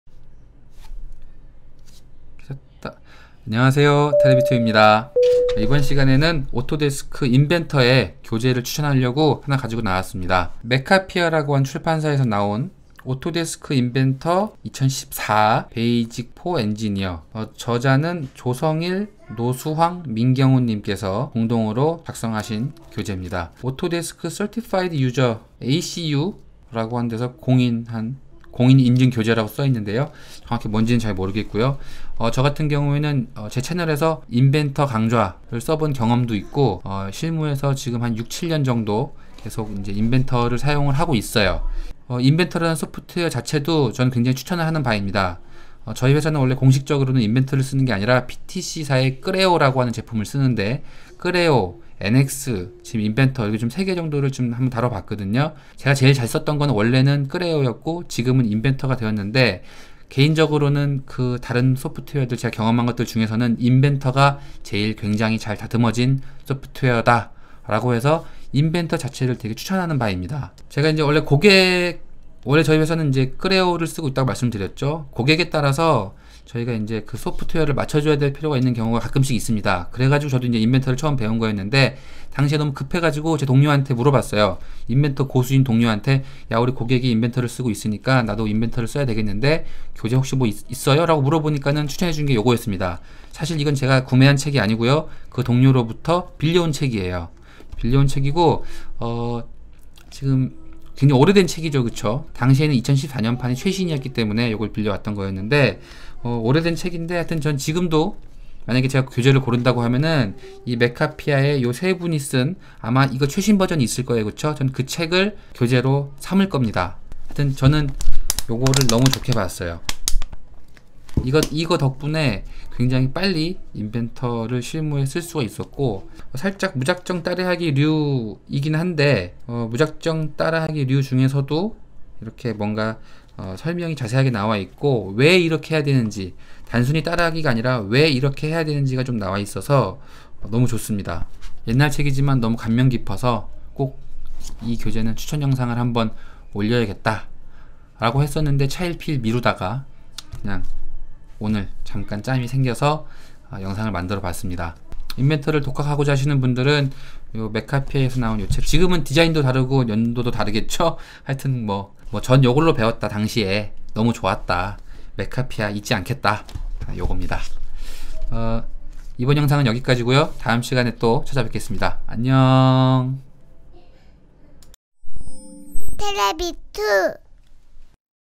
독학용 인벤터 교재 추천 (광고아님)
몇 년이 흘렀지만, 너무 좋았던 책이라 소개합니다.

메카피아의 Autodesk Inventor 20XX : Basic for Engineer

동일 출판사의 최신판도 있네요. 공동 저자 한 분 빼고는 전부 바뀌신거 같은데 그래도 내용은 여전히 좋을 것으로 기대됩니다.
독학하시는 분들에게 추천이에요.